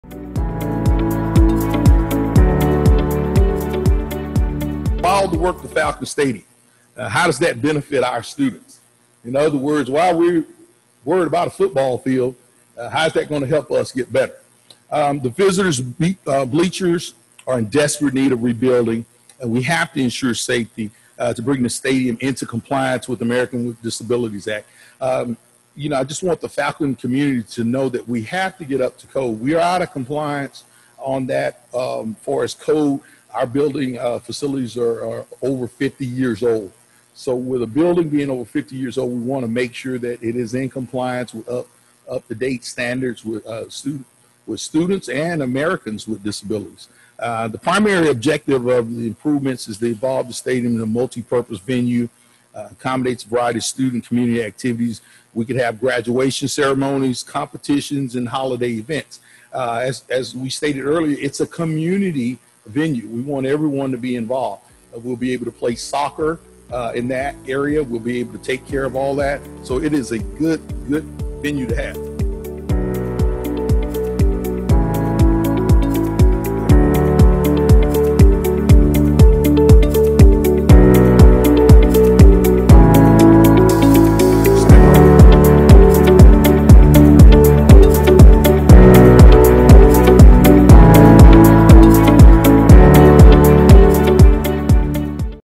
10/13/20 "Bond Facts at Five": Proposed Falcon Stadium Improvements
For the 10/13 "Bond Facts at Five", you will hear a message from RHS Principal Tony Runnels regarding the proposed improvements for Falcon Stadium, such as safety upgrades and conversion to a multi-purpose community facility. Early voting is in full swing!